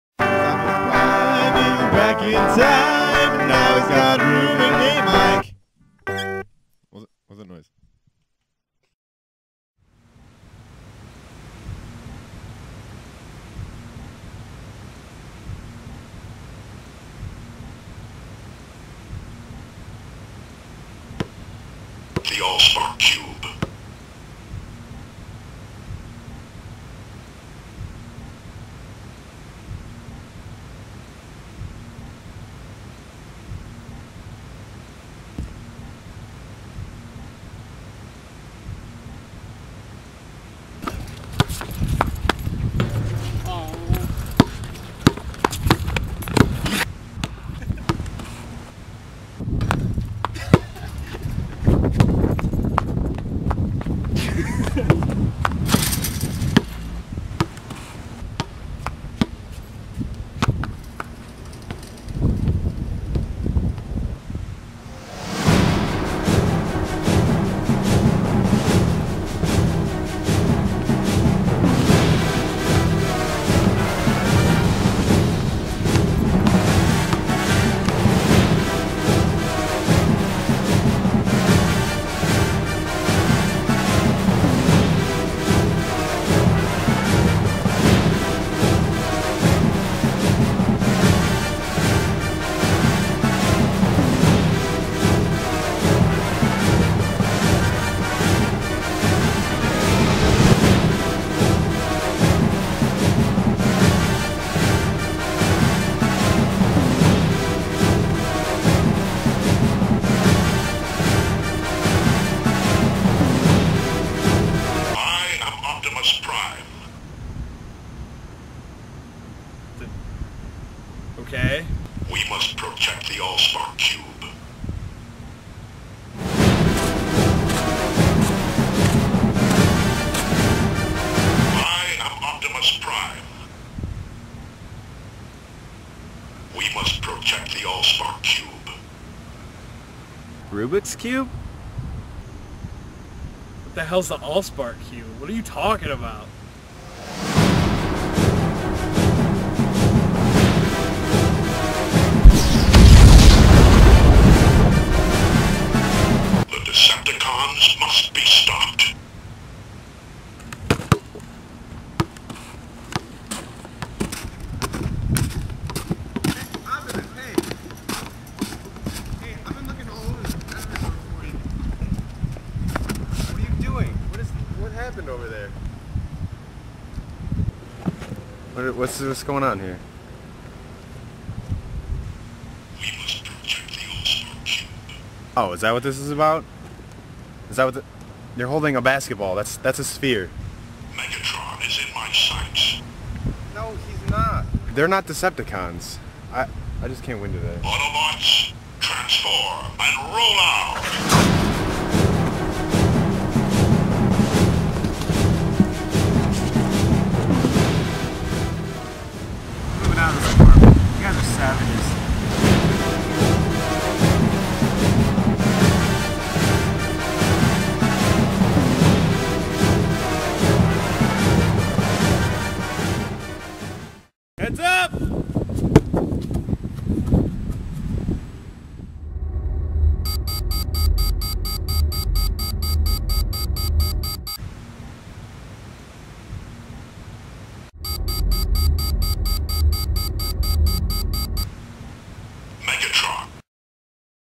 Optimus Prime and Mike - Episode 3
Optimus Prime went back in time and now he's got a roommate named Mike